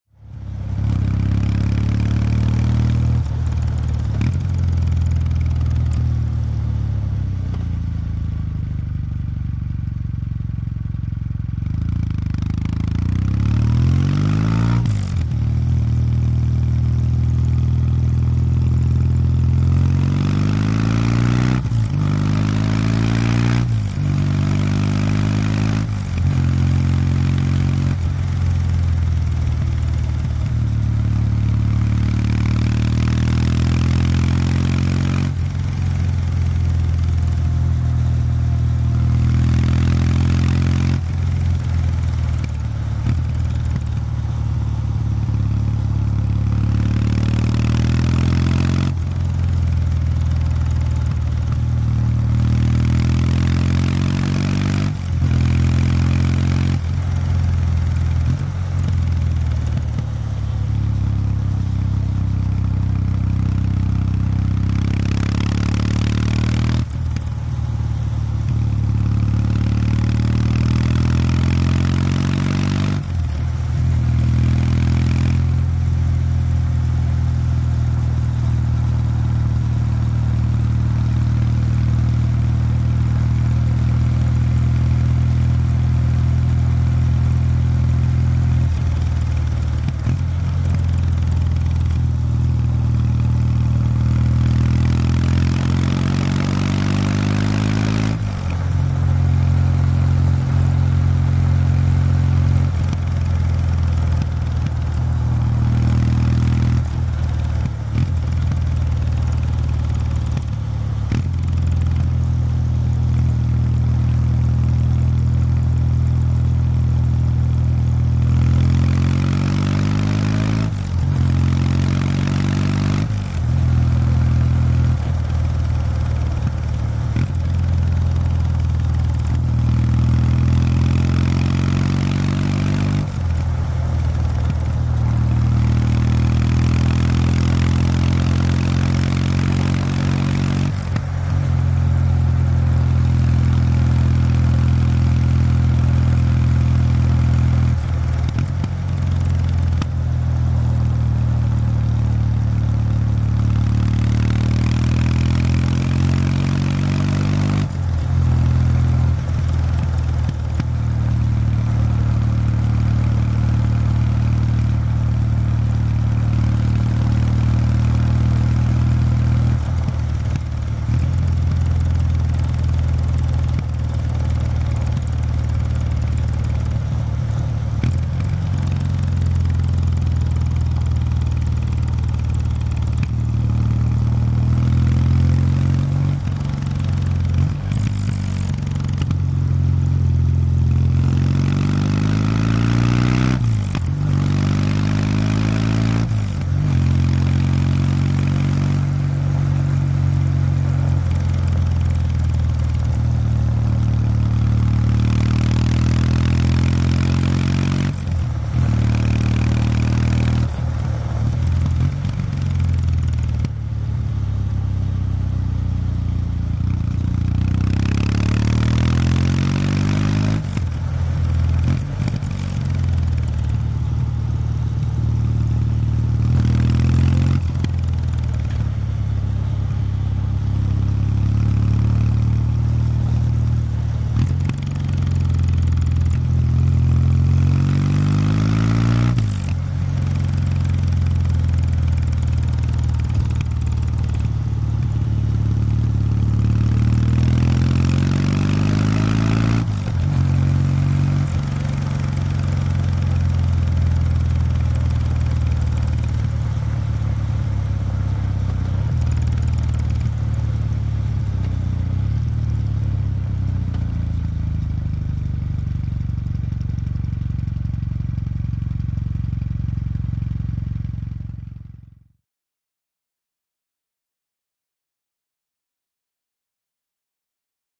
W800 Street リラックスしたエンジン音｜新潟県道433号
里山を散歩中、忽然と現れた快走路。
新潟県道433号ですが、400番代とは思えない快走路です。
今回はリラックスしたエンジン音でゆったり流してみました。
※Exhaust sound only

●Yas!のチャンネルではバイクの音にこだわった動画をアップしてます。
バイクの音をBGMに四季折々の道の表情をお楽しみください。

◉Equipment
Kawasaki W800 Street 2019 (stock exhaust)
SONY HDR-AS300
TASCAM DR-40X